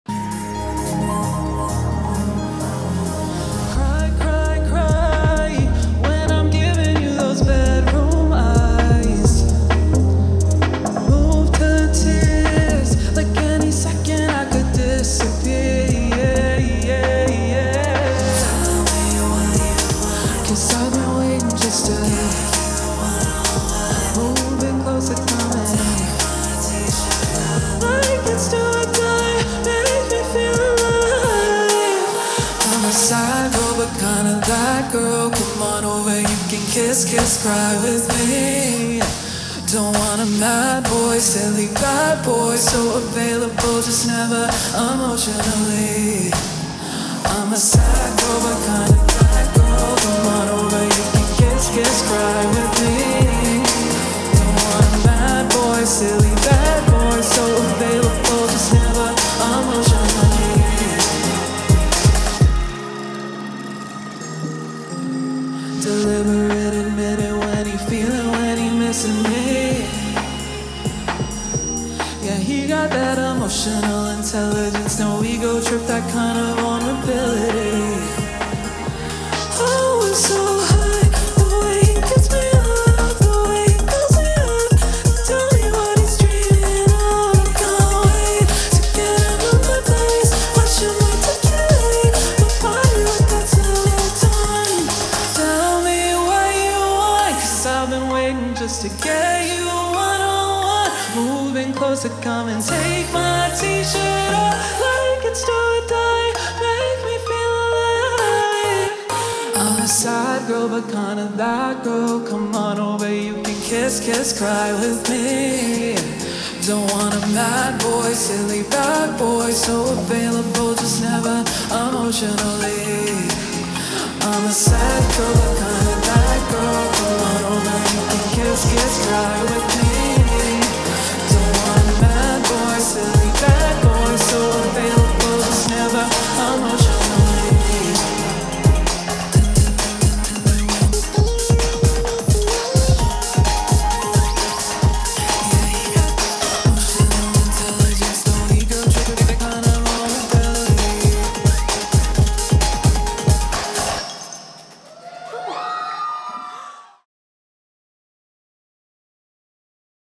Talltale at Festival du Voyageur 2024 - Canada's Walk of Fame RBC Emerging Musician Program
2023 Canada's Walk of Fame RBC Emerging Musician Program 2nd Prize Winner Talltale performs at Festival du Voyageur in Winnipeg on February 18, 2024.

Video: The Quigley Dream Company